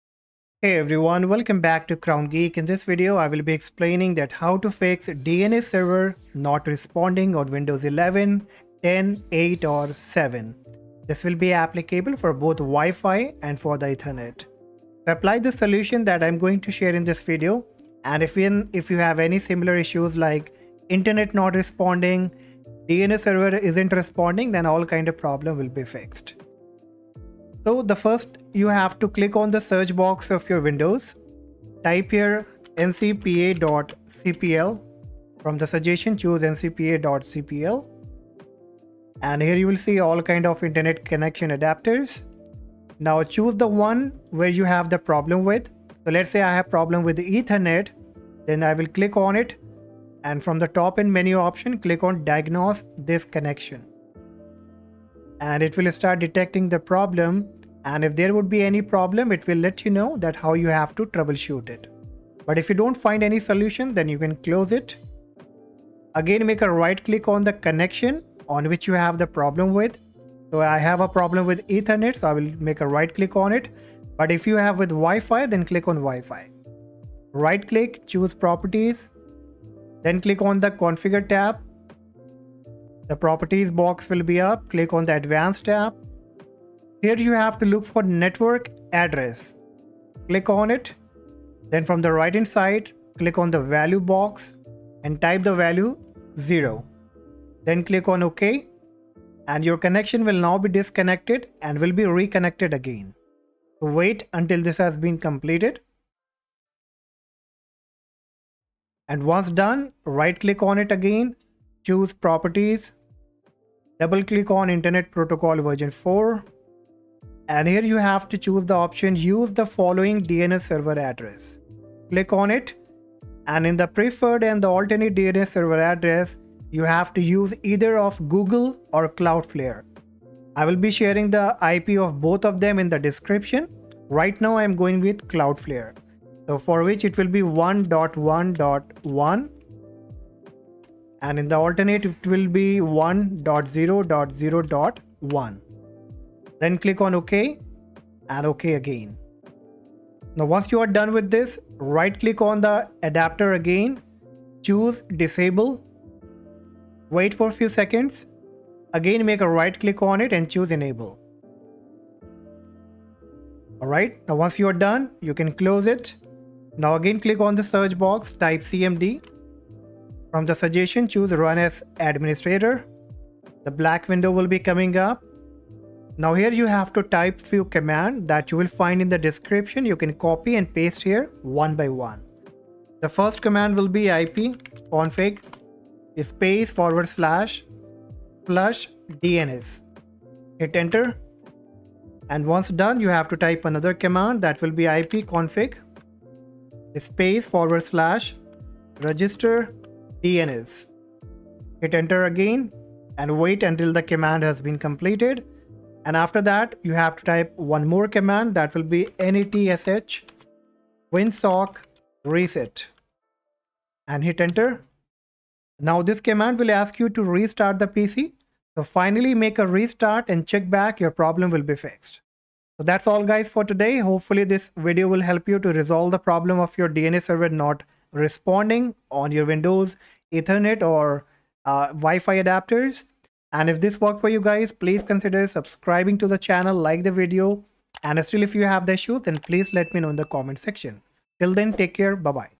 How to Fix DNS Server Not Responding On Windows 11/10/7/ | Wi Fi or Ethernet Connection (2022)
This video will help you to fix DNS issues like DNS isn't responding or Internet not responding issues on Windows PC and Laptop.

►Google DNS
Preferred Server Address - 8.8.8.8
Alternate Server Address - 8.8.4.4

►Cloudflare DNS
Preferred Server Address - 1.1.1.1
Alternate Server Address - 1.0.0.1

►CMD Network Commands
ipconfig /flushdns
ipconfig /registerdns

►Timestamps
00:00 - Intro
00:30 - Solution 1  Diagnose Network Connection
01:10 - Solution 2 Change Network Address Value
01:49 - Solution 3  Change DNS Server Address
02:30 - Solution 4  Disable/Enable Network Adapter
02:45 - Solution 5  Run Network Commands

☑ Watched the video!
☐ Liked?

►(Related Queries)
How do you fix a DNS server that's not responding?
How do you reset your DNS server?